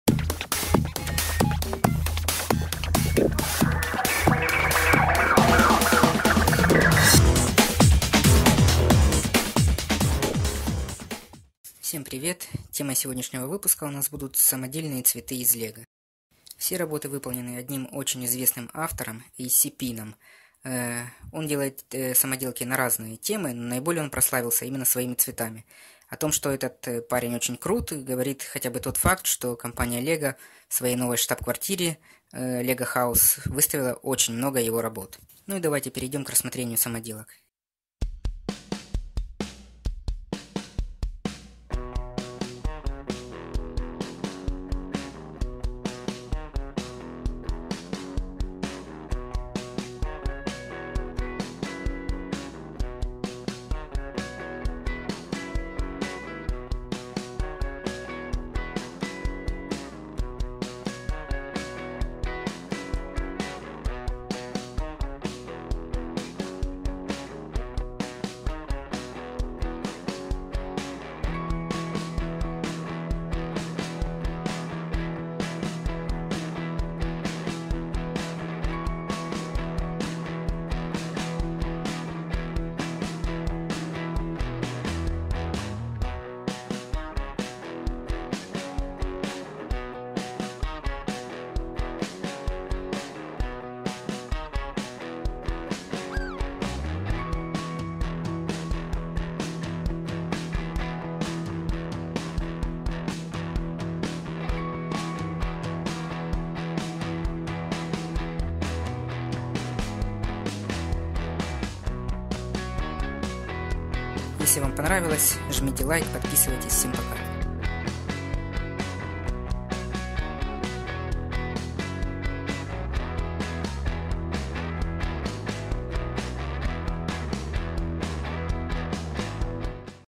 Цветы из Лего.
Самодельные цветы из Лего от одного из самых известных авторов AC Pin.